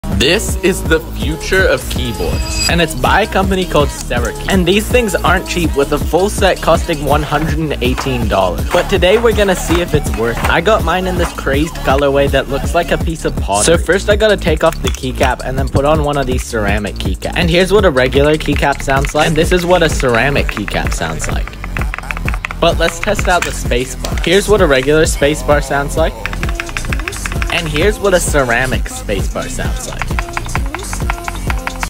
The Future of Keyboards
Ceramic keycap review by a company called Cerakey.